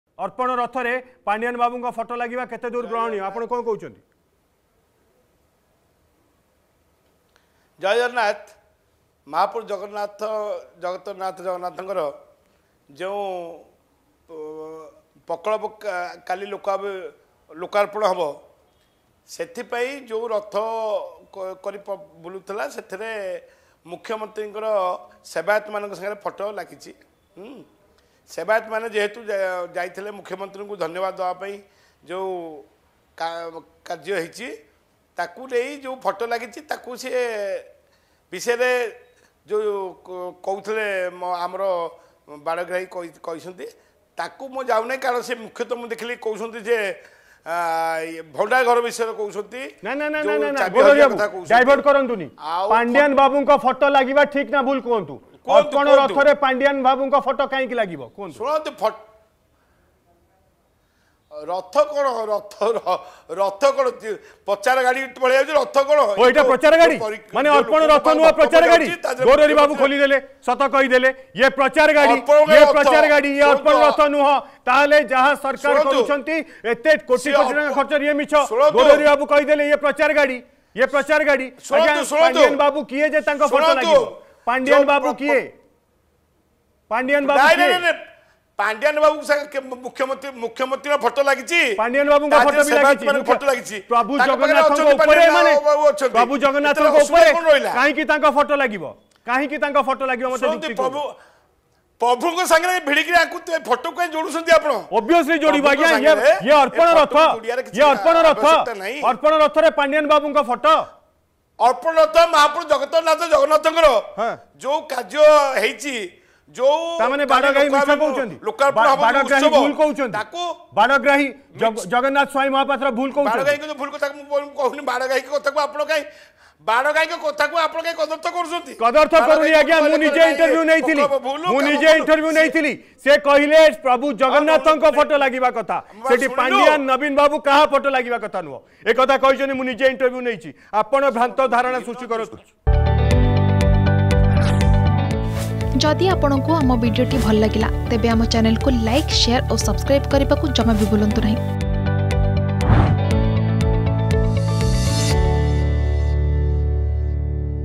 ଅର୍ପଣ ରଥରେ ମହାପ୍ରଭୁଙ୍କ ଫୋଟ ଜାଗାରେ ଲାଗିଛି ରାଜନୈତିକ ଦଳର ଫୋଟଚିତ୍ର || ParikramaPrakalp controversy
ଅର୍ପଣ ରଥରେ ମହାପ୍ରଭୁଙ୍କ ଫୋଟ ଜାଗାରେ ଲାଗିଛି ରାଜନୈତିକ ଦଳର ଫୋଟଚିତ୍ର ।